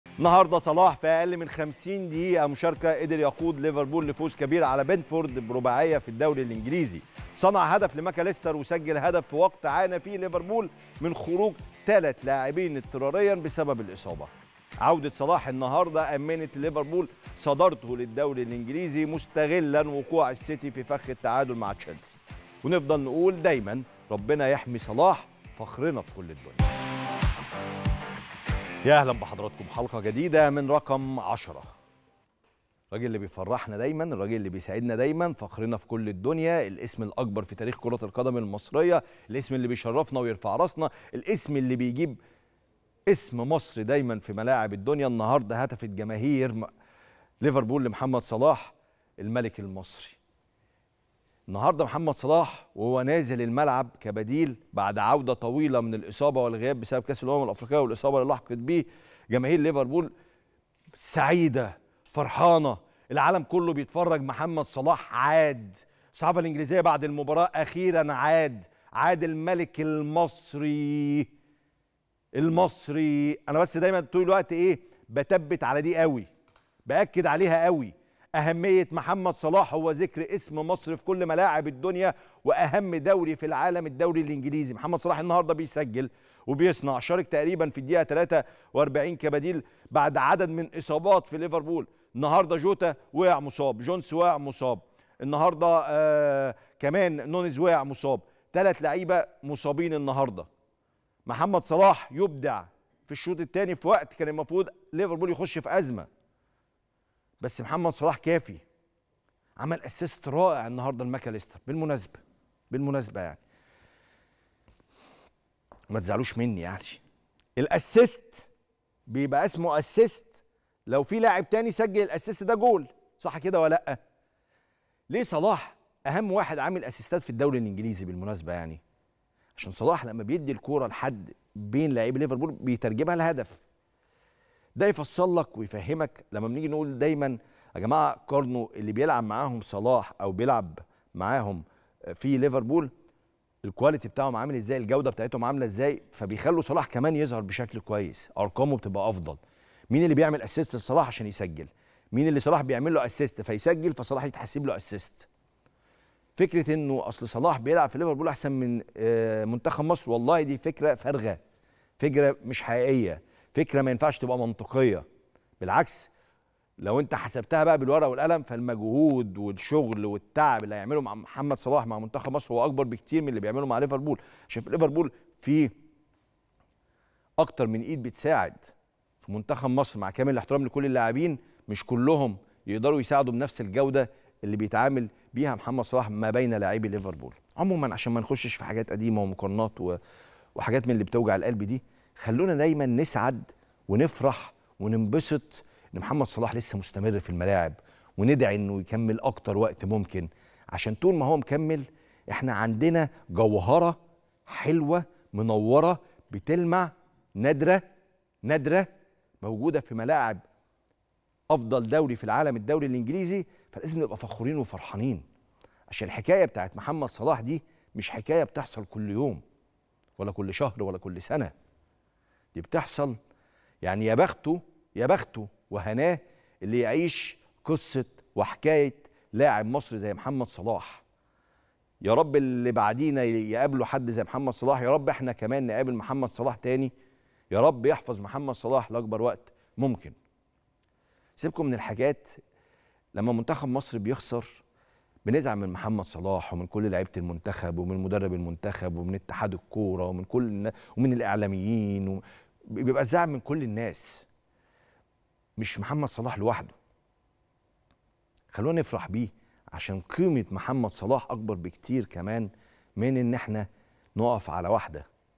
عاد الملك👑.. مقدمة قوية من كريم رمزي على تألق صلاح بعد عودته: شوفوا اللاعيبة اللي بتلعب مع صلاح 🔥
عاد الملك👑.. مقدمة قوية من كريم رمزي على تألق صلاح بعد عودته من الإصابة: شوفوا اللاعيبة اللي بتلعب مع صلاح 🔥